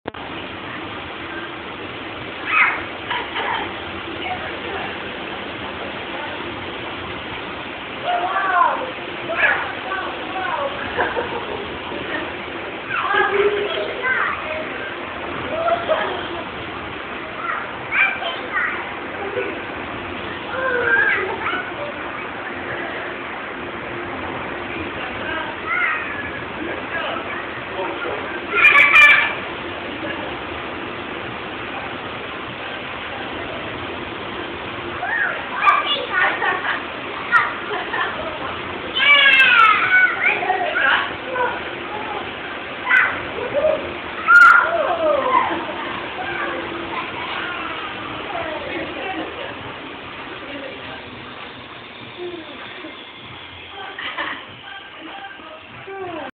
23.08.07 freddys (sky diving) at airkix wind tunnel
went to airkix wind tunnel for an 18th bday taken by phone